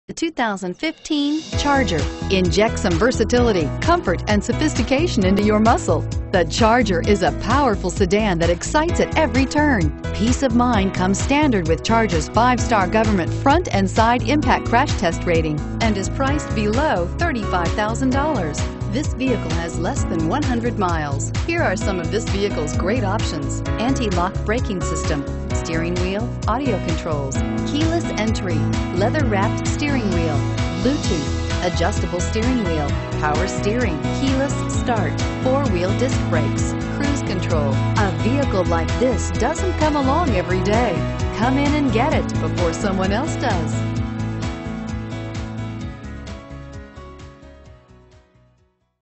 2015 Dodge Charger FH866864
2015 DODGE CHARGER Paramus, NJ
Stock# FH866864

Hyundai Of Paramus
234 Route 4 East

Pick up this Granite Crystal Metallic Clearcoat 2015 Dodge Charger, available today. Featuring Automatic transmission, it has 25 miles. This could be the one you've been searching for.